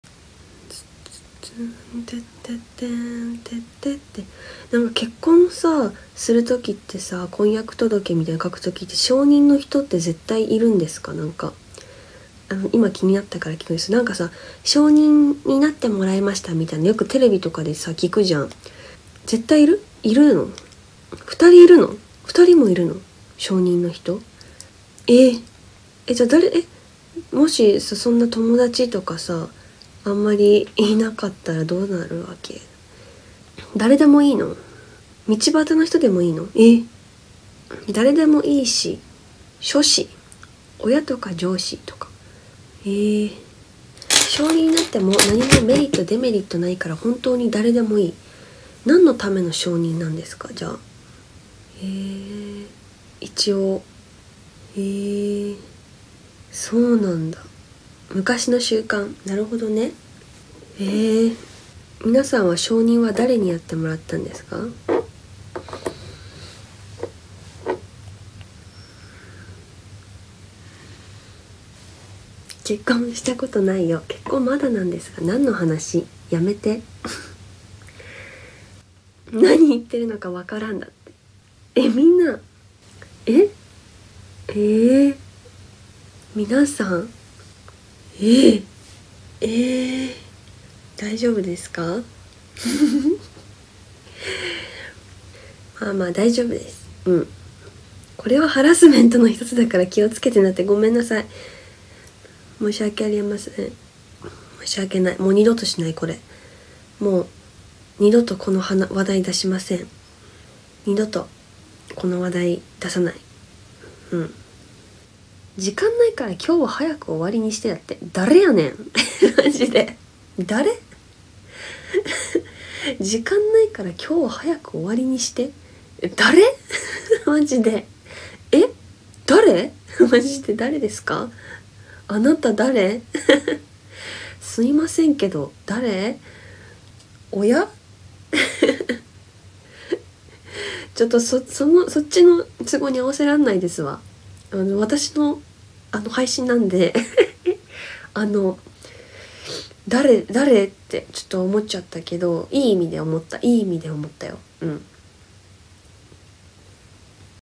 ファンに強烈なハラスメントをしてしまい謝罪 AKB48 小田えりな
AKB48 チーム8 小田えりな

2022年11月11日 SHOWROOM配信より